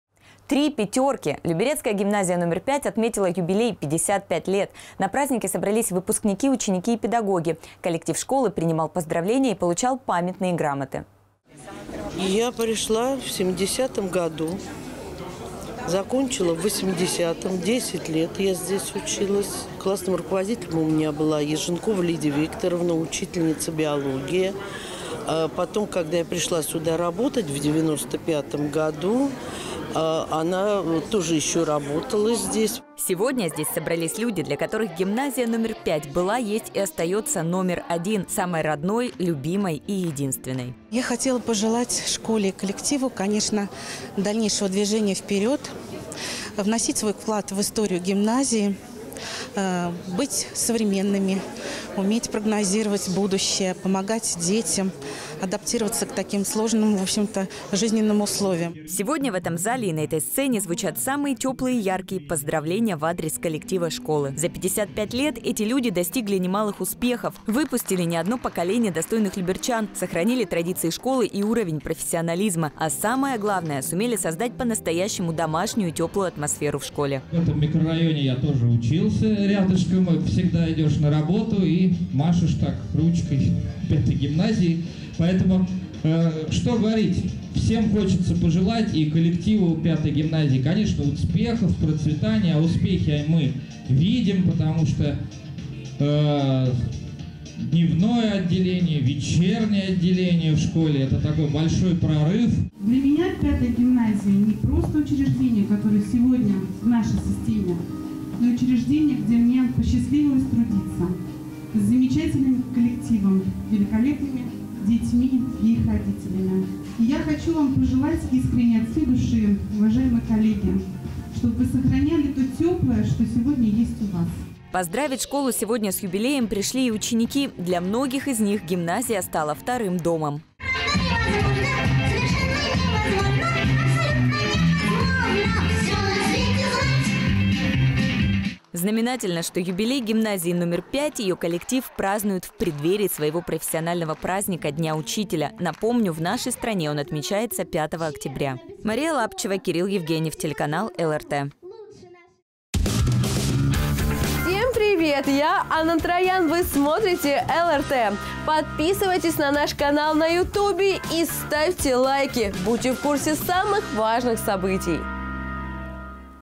55 лет исполнилось гимназии №5
Три пятерки! Люберецкая гимназия No5 отметила юбилей – 55 лет! На празднике собрались выпускники, ученики и педагоги. Коллектив школы принимал поздравления и получал памятные грамоты.

Светлана Колобаева, учитель математики гимназии No5, г. Люберцы:
«Я пришла в 1970м году. Закончила в 1980м. 10 лет я здесь училась. Классным руководителем у меня была Еженкова Лидия Викторовна, учительница биологии. Потом когда я пришла сюда работать в 95м году, она тоже еще работала здесь».

Сегодня здесь собрались люди, для которых гимназия No5 была, есть и остается номер один. Самой родной и любимой и единственной.

Лариса Левушкина, директор гимназии No5, г. Люберцы:
«Я хотела бы пожелать школе и коллективу дальнейшего движения вперед, вносить свой вклад в историю гимназии, быть современными, уметь прогнозировать будущее, помогать детям адаптироваться к таким трудным жизненным условиям».

Сегодня в этом зале и на этой сцене звучат самые теплые и яркие поздравления в адрес коллектива школы. За 55 лет эти люди достигли немалых успехов: выпустили не одно поколение достойных люберчан, сохранили традиции школы и уровень профессионализма. А самое главное – сумели создать по-настоящему домашнюю и теплую атмосферу в школе.

Борис Новиков, начальник управления по работе с молодежью г.о. Люберцы:
"В этом микрорайоне я тоже учился рядышком. Всегда идешь на работу и машешь так ручкой пятой гимназии». Поэтому, что говорить? Конечно, коллективу пятой гимназии хочется пожелать и успехов, и процветания. А успехи мы видим, потому что дневное отделение в школе, вечернее отделение в школе, это такой большой прорыв".

Светлана Яковенко, заместитель начальника управления образованием г.о. Люберцы:
"Для меня пятая гимназия не просто учреждение, которое сегодня есть в нашей системе, но и учреждение, где мне посчастливилось трудиться, с замечательным коллективом, великолепными детьми и их родителями. И я хочу вам пожелать искренне, от всей души, уважаемые коллеги, чтобы вы сохраняли то теплое, что сегодня есть у вас".

Поздравить школу сегодня с юбилеем пришли и ученики. Для многих и них гимназия стала вторым домом.Знаменательно, что юбилей родной школы коллектив гимназии No5 празднует в преддверии своего профессионального праздника – Дня учителя. Напомню, в нашей стране он отмечается 5 октября.

Мария Лапычева, Кирилл Евгеньев